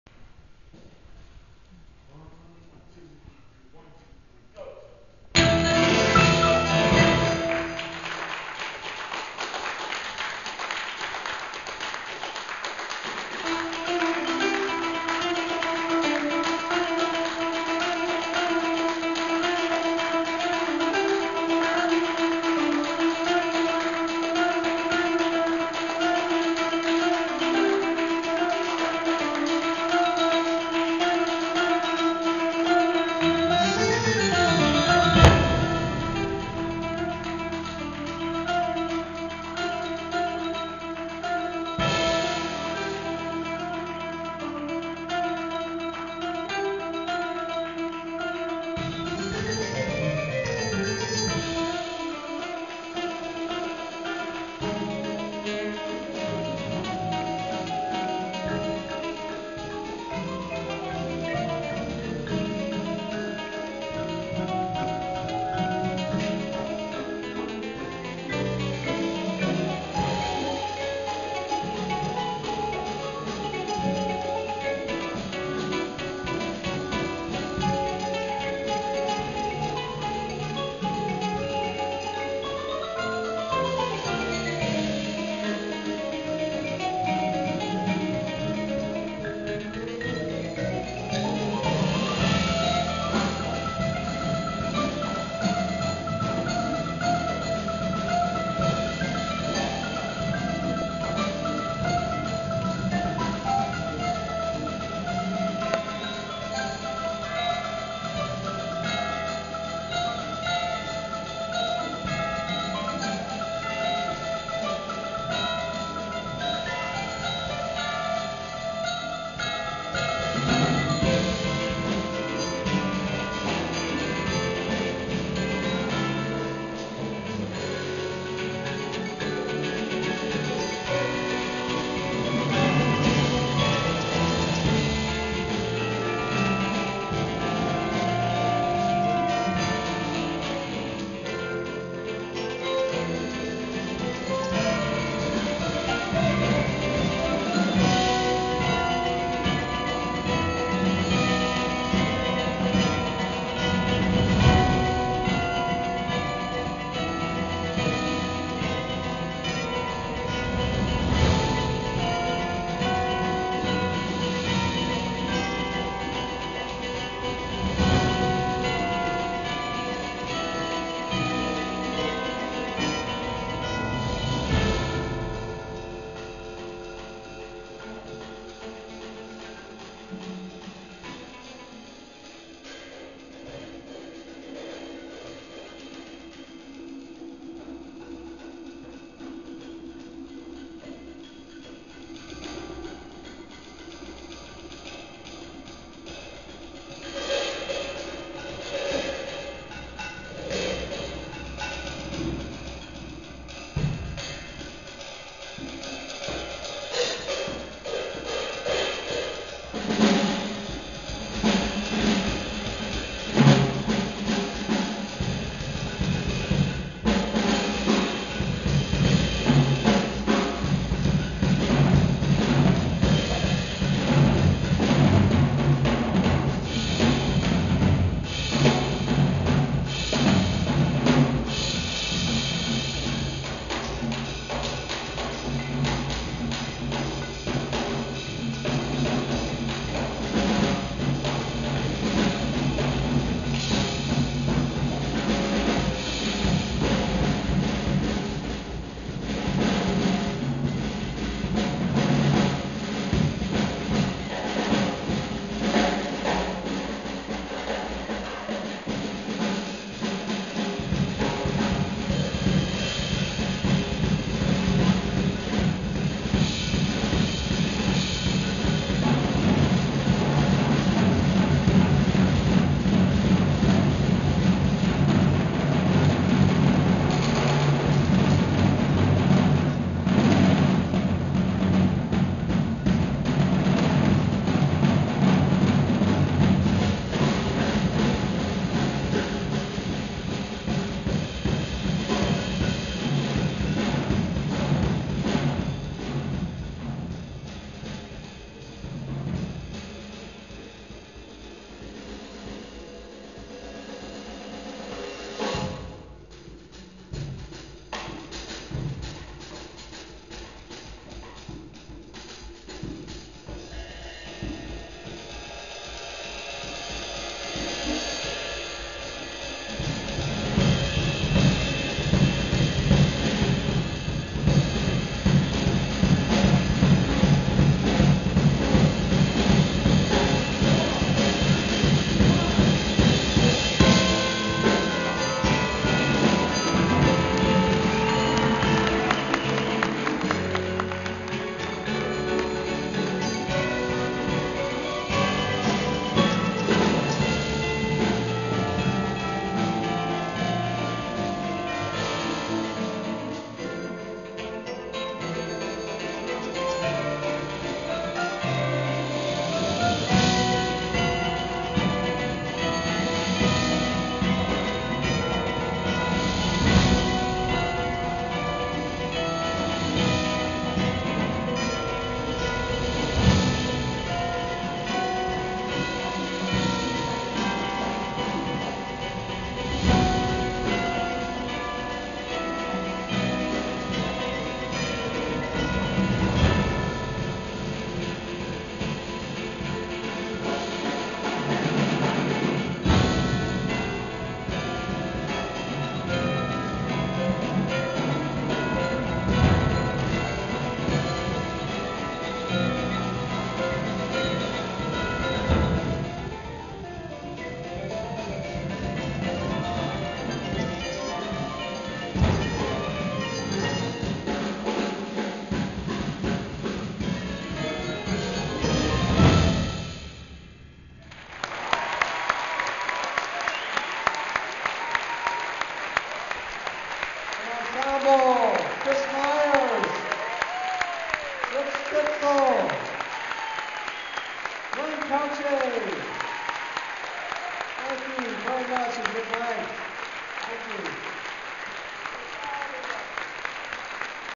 Elmhurst Percussion Recital 03/08/07 Elmhurst College - Elmhurst, IL
Percussion Drum Clinic
Hammerschmidt Memorial Chapel
Elmhurst College, Elmhurst, IL

Kris Myers - drums
Glen Kotche - percussion
Ritch Stitzel - percussion
Elmhurst Percussion Ensemble
Chris Siebold - guitar

sorry, not a very good vantage point.  Kotche and Stitzel are directly behind guitarist Siebold. Myers is behind the amplifier on the left.